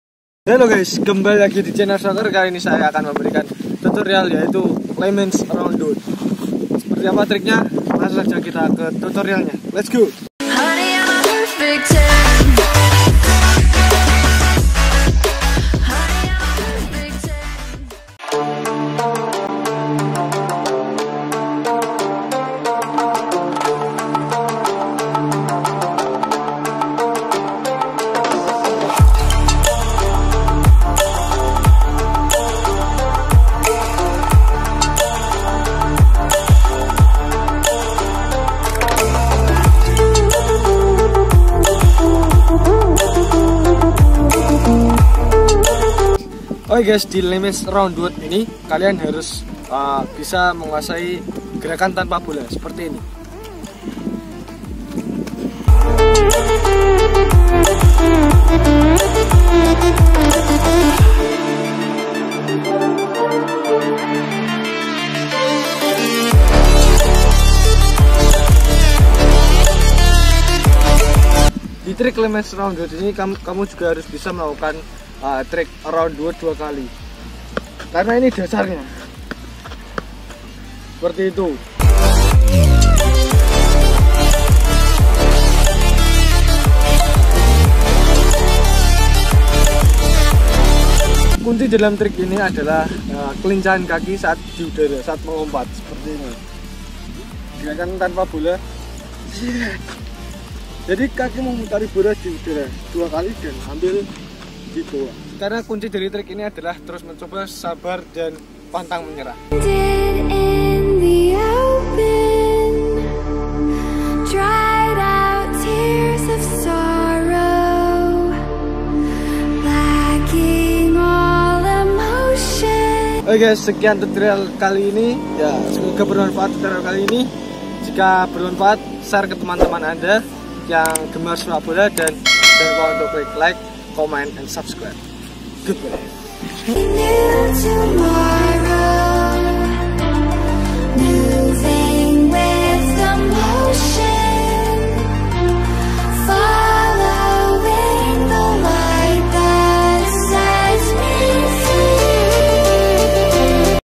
Lemmens Around The World Tutorial Trick Freestyle Sepak Bola Indonesia
Lemmens around the world atau biasa di sebut latw adalah sebuah trick freestyle yang dibilang sulit, namun ketika kita sudah tau tehnik tehnik nya akan lebih mudah untuk mempelajari dan melakukan trick ini yaitu lemmens around the world. dan video kali ini berisi tentang tehnik tehnik trick lemmens around the world atau latw, dan juga tutorial trick tersebut.